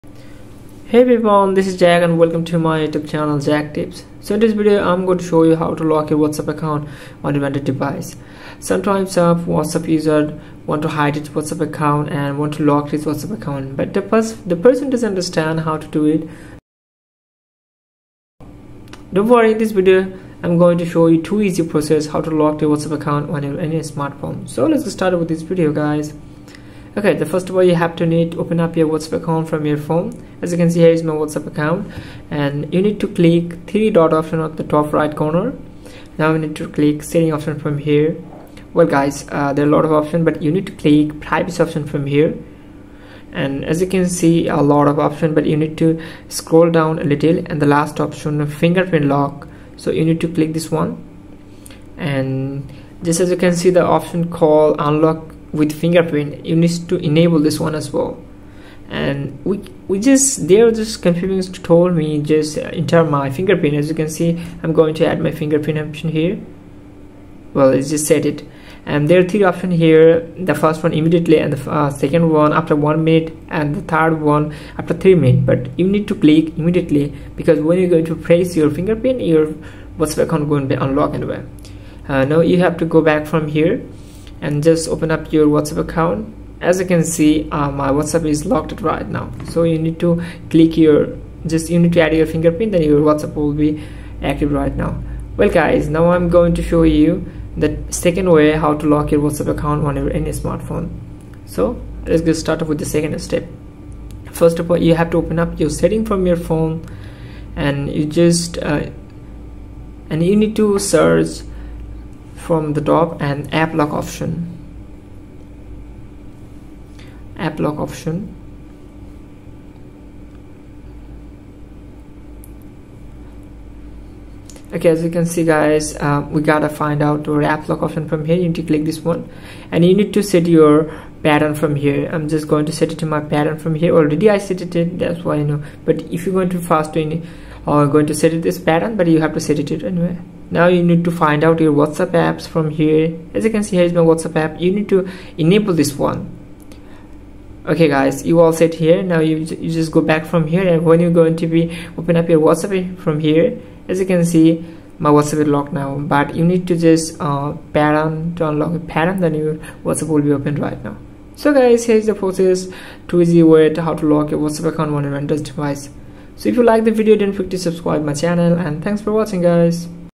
How To Lock WhatsApp Account | How To Set Password In WhatsApp 2023
If you want to know how to lock your WhatsApp Account or if you want to make secure your WhatsApp Account. then you must have to be watch this full video to know how to lock your WhatsApp Account with out any tired party apps.
So, In this video, I will show you how to lock your WhatsApp Account lock or to make private from your WhatsApp Account on any smart phone.